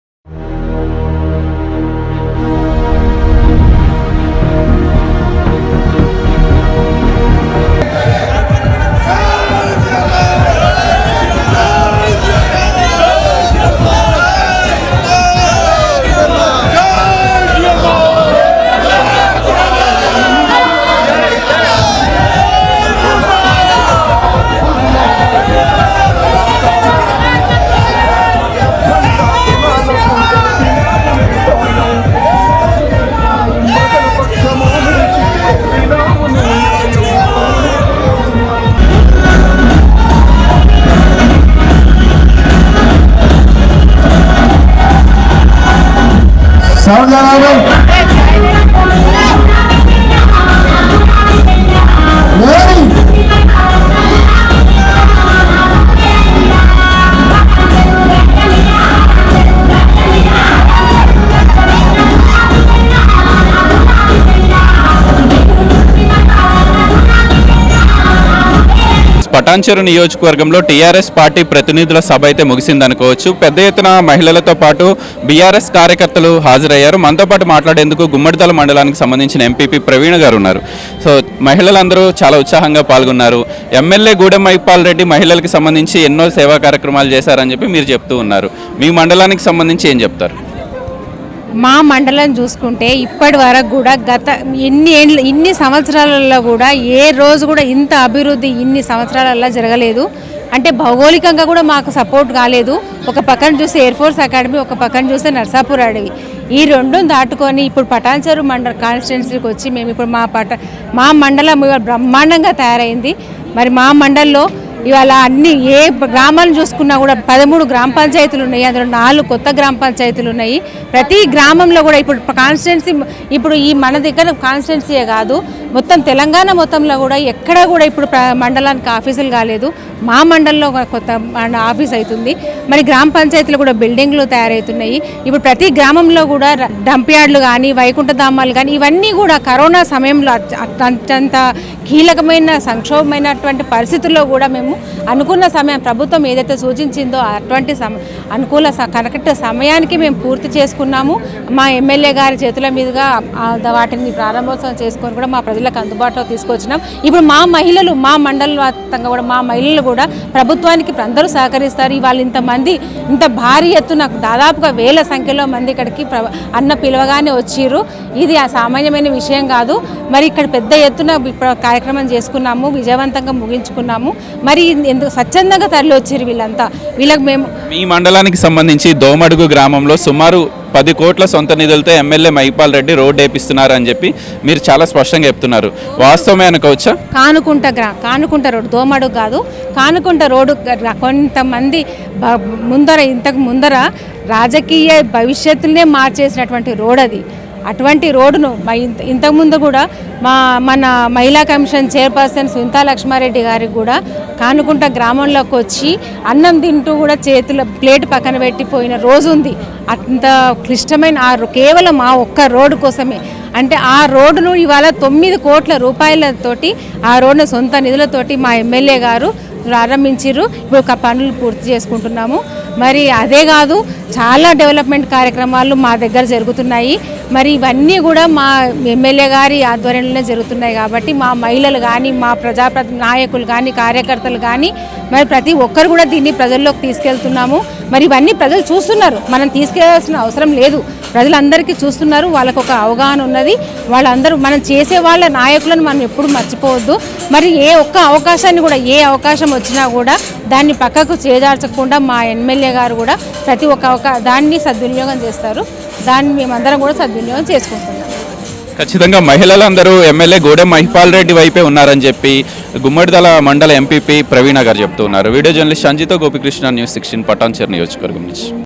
Patancheru : గూడెం మహిపాల్ రెడ్డి వ్యక్తి కాదు నియోజకవర్గానికి ఓ శక్తి | Gummadidal MPP Praveena
Patancheru : గూడెం మహిపాల్ రెడ్డి వ్యక్తి కాదు నియోజకవర్గానికి ఓ శక్తి  | Gummadidal MPP Praveena Face to Face | NEWS16 Telugu

Neelam Madhu Padi Puja : దమ్మున్న మాస్ లీడర్ నీలం మధు | BRS MLC Bandaru Prakash Mudiraj Face to Face

 / news16telugu​.  .